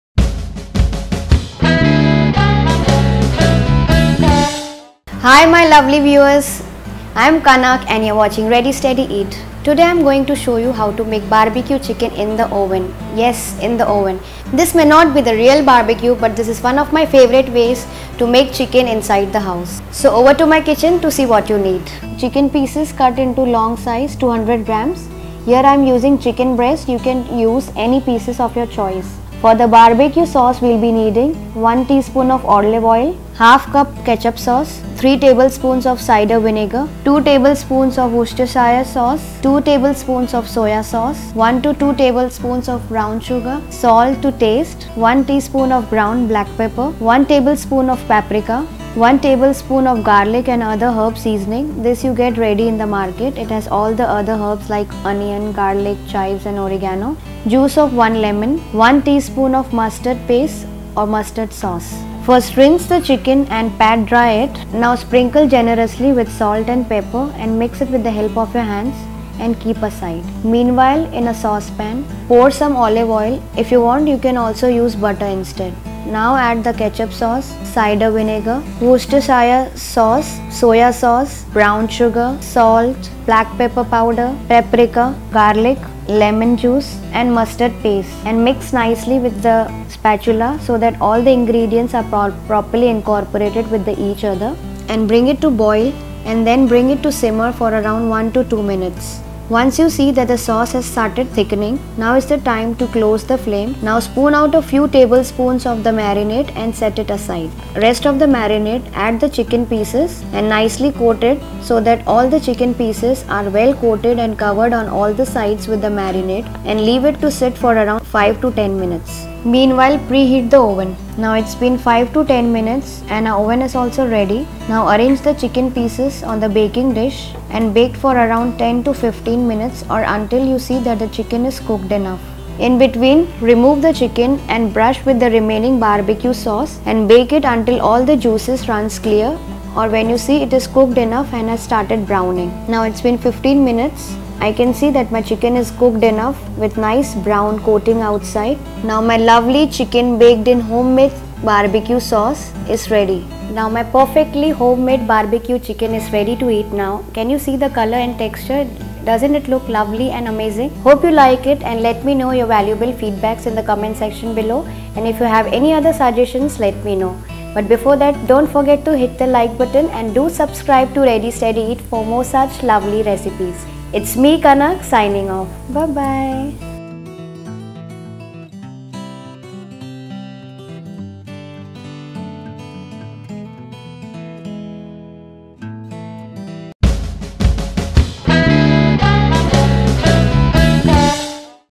Barbecue Chicken Recipe
Barbecue Chicken - Easy Barbecue Chicken Recipe - APPETIZER
Watch Kanak's easy baked barbecued chicken with homemade barbecue sauce and chicken pieces.

If you like this recipe, Hit Like Button and provide your feedback in the comments section below!

Thanks :)